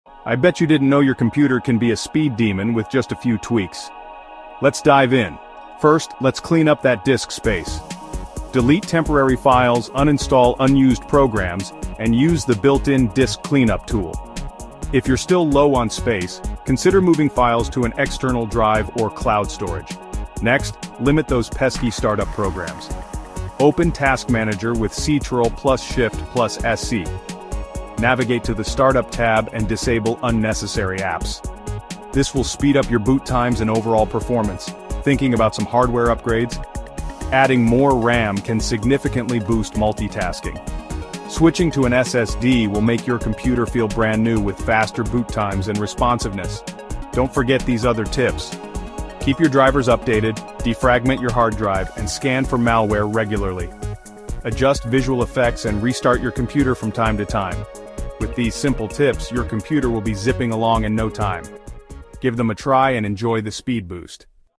Boost Your PC Speed Easy Tips!
ev up your computer's performance with our easy-to-follow tips! In this video, we’ll show you how to boost your PC speed by cleaning up disk space, uninstalling unused programs, and limiting startup applications. Discover the power of hardware upgrades like adding RAM or switching to an SSD for a game-changing performance boost. Plus, learn important maintenance tasks such as updating drivers, defragmenting your hard drive, and scanning for malware to keep your system running smoothly. Don't let a sluggish computer hold you back!